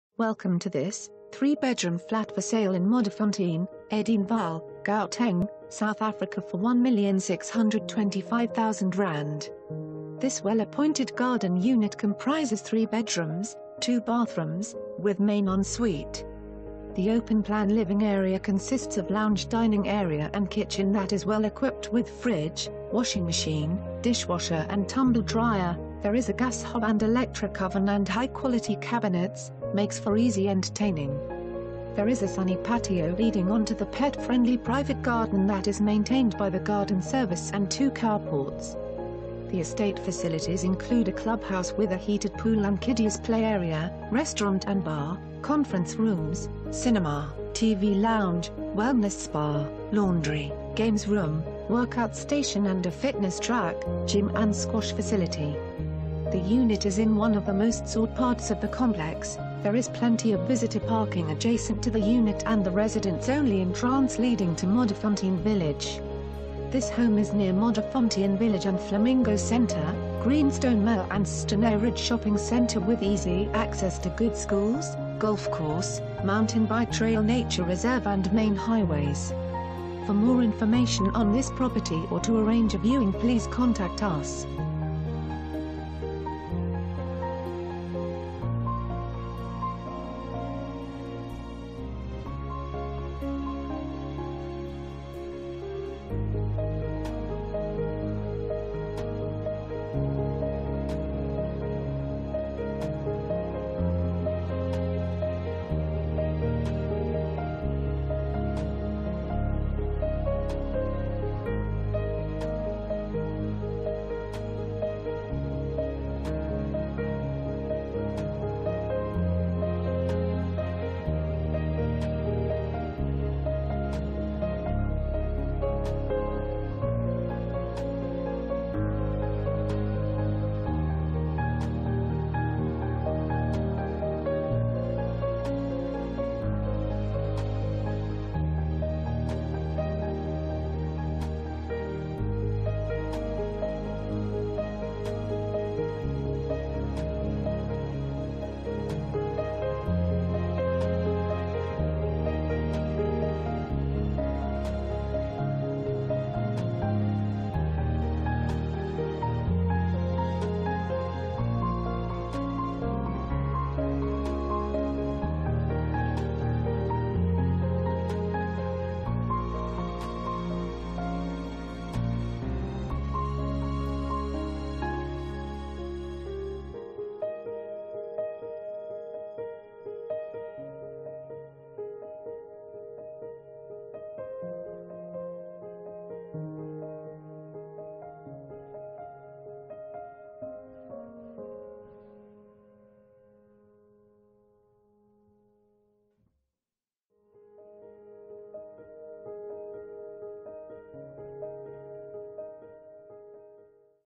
3 Bedroom Flat For Sale in Modderfontein, Edenvale, Gauteng, South Africa for ZAR 1,625,000
This well-appointed garden unit comprises 3 Bedrooms, 2 Bathrooms, with main en-suite. The open plan living area consists of lounge/dining area and kitchen that is well equipped with Fridge, Washing Machine, Dishwasher and Tumble Dryer, there is a gas hob and electric oven and high-quality cabinets, makes for easy entertainin...